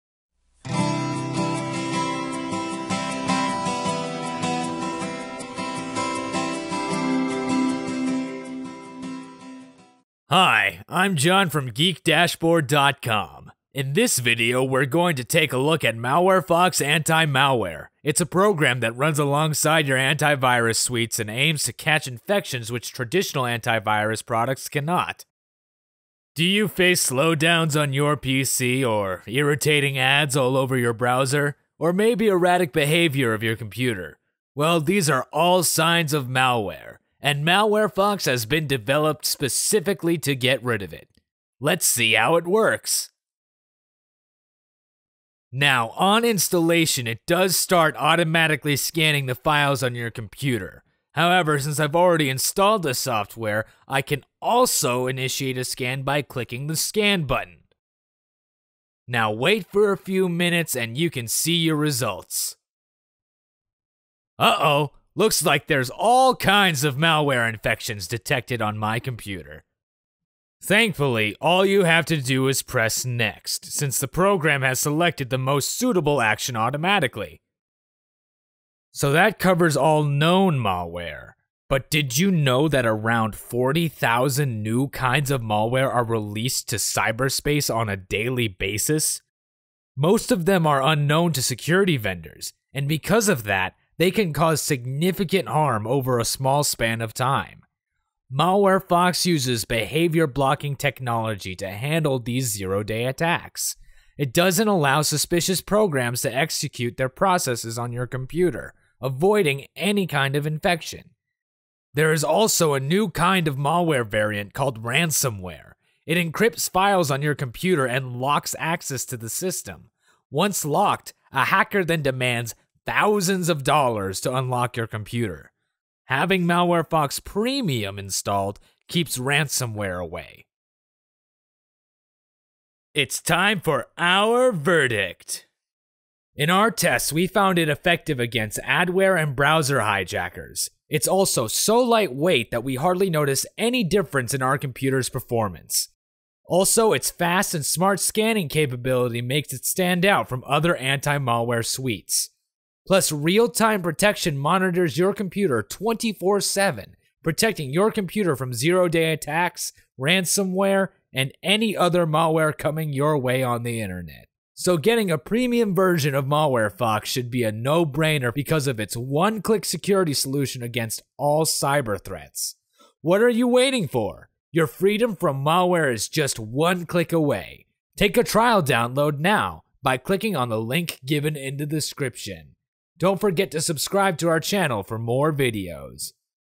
Prevent Your Computer from All Kinds of Malware Including Ransomware - MalwareFox Review
MalwareFox AntiMalware helps your computer from being affected with any kind of Malware. Here is why you should try MalwareFox Premium and keep ransomware attacks at bay.